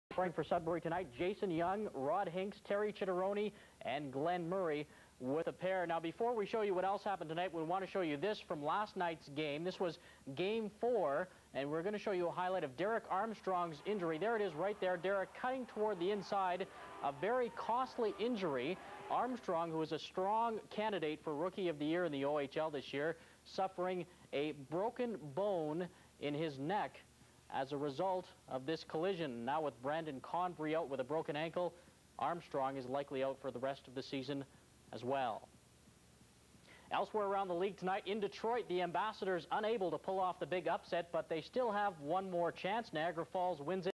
OHL Mar. 22, 1992 Sean Brown,OSH v Derek Armstrong,SUD (hit) (HL) Oshawa Generals Sudbury Wolves
OHL March 22, 1992 Sean Brown,OSH v Derek Armstrong,SUD (hit) (HL)